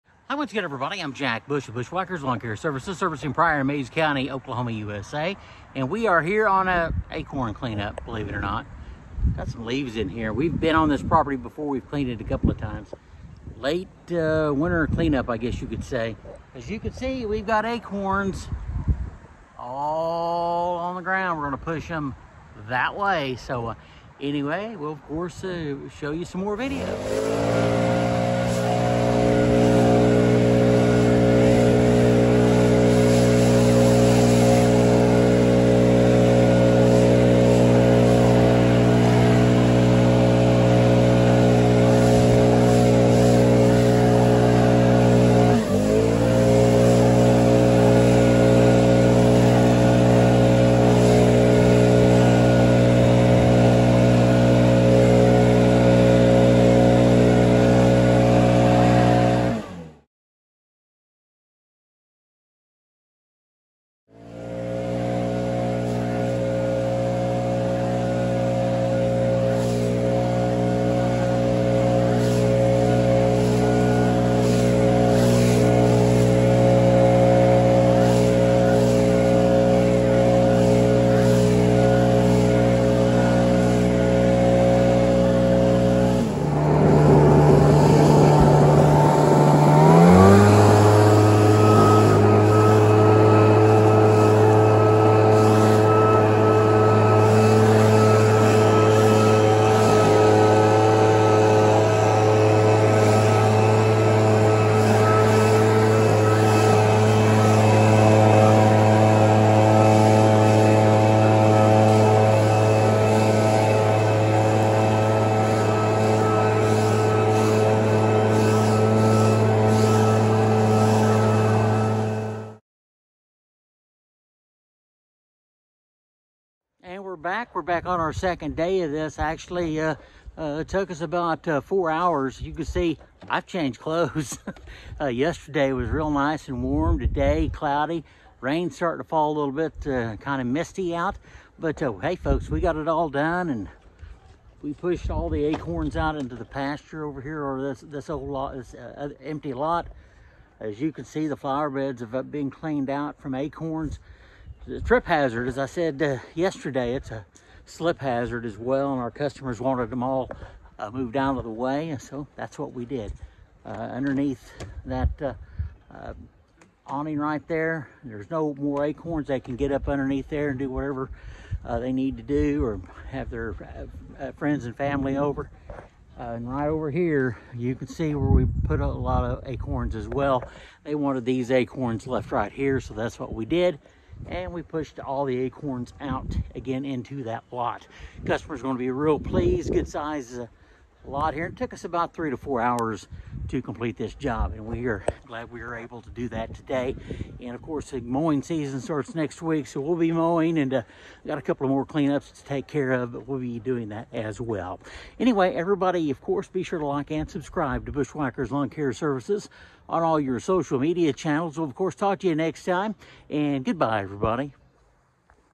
Acorn Cleanup. Yes, it’s a real thing.
One of our customers called and needed a large amount of acorns removed from the back yard. So many acorns in fact that you could slip while walking across the yard.